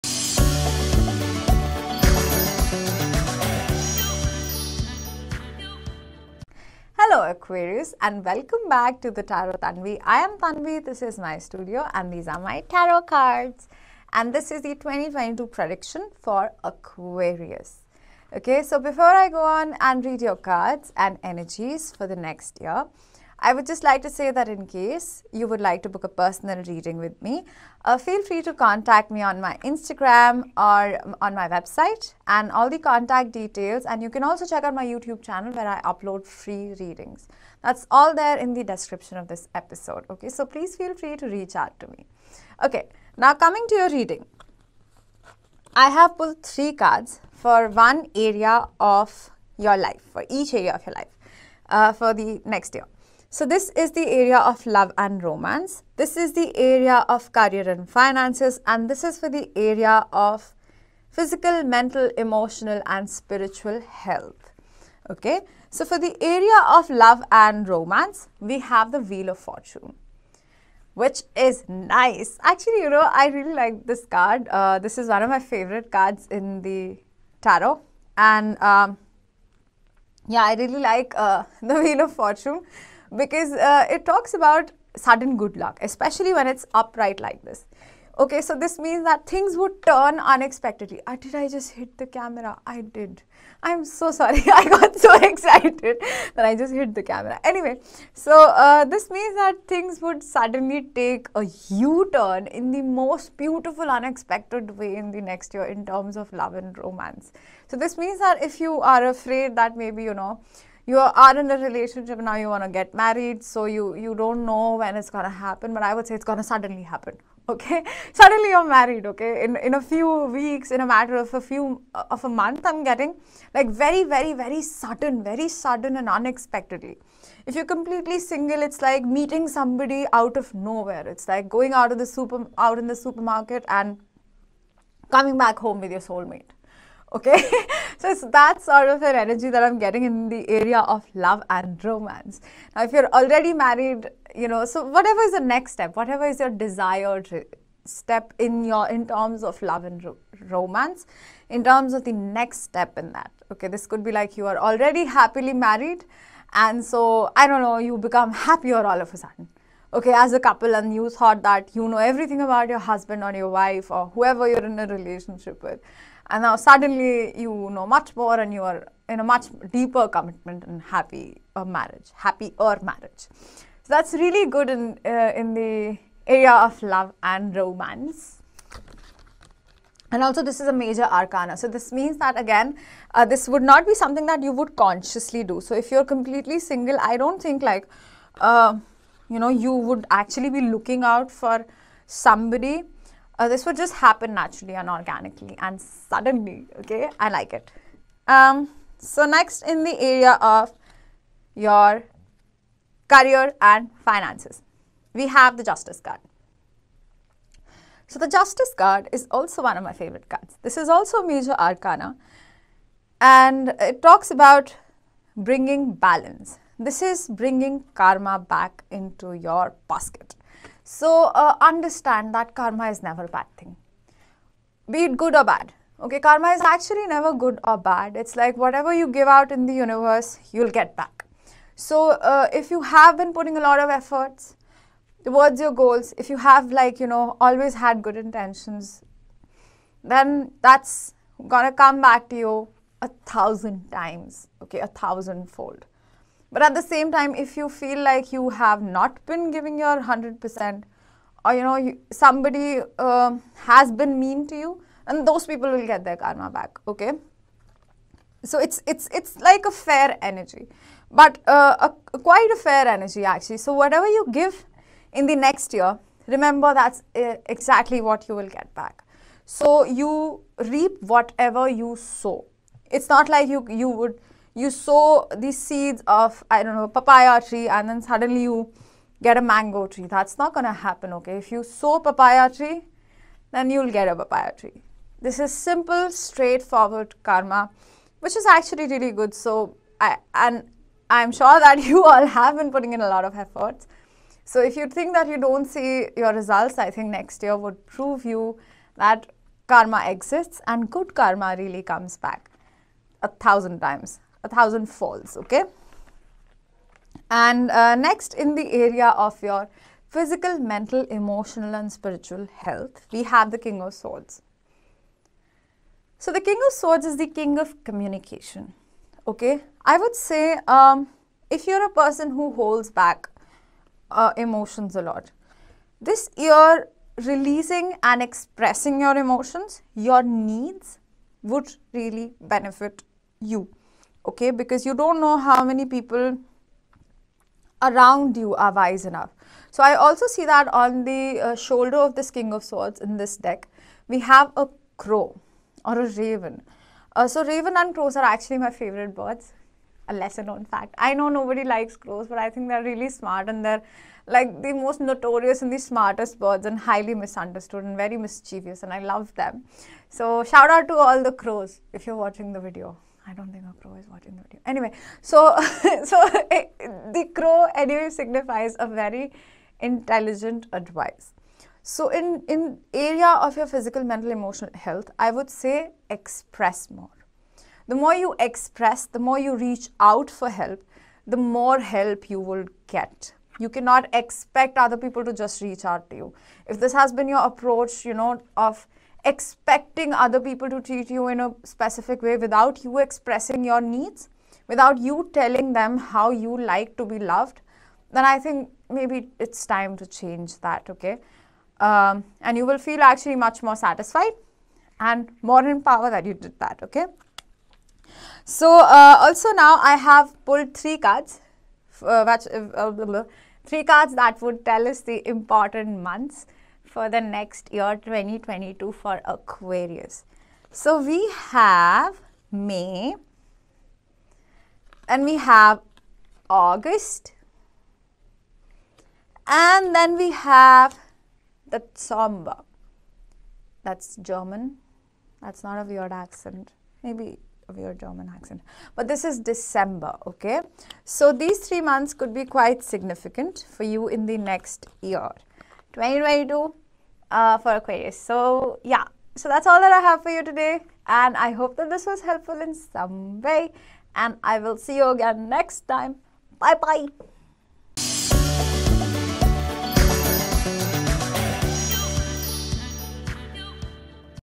Aquarius predictions for 2022: Prepare for a major U-turn | OneIndia News
Tarot Tanvi predicts your year for 2022. Aquarius, there is a major change coming up ahead for you. Watch to know more.